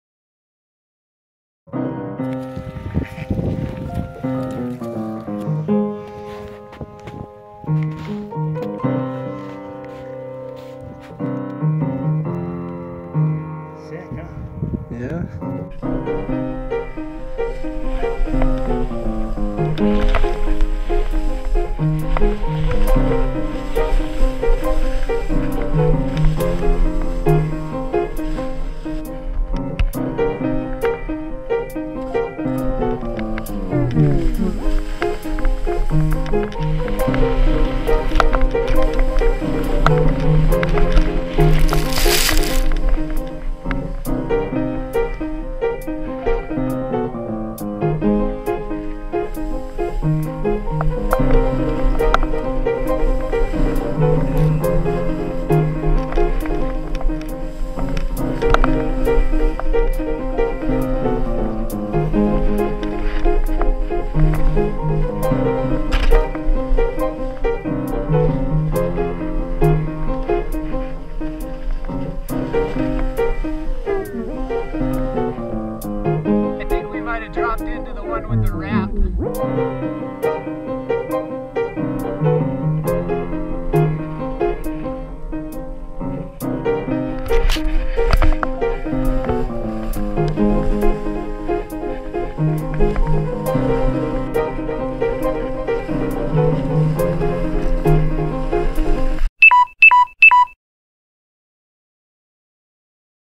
West Twin Couloir
Around half of the Minor couloir (not the "Pencil Thin") of the West Twin in the Chugach.  Since my battery died, lets pretend I straight lined the rest of it.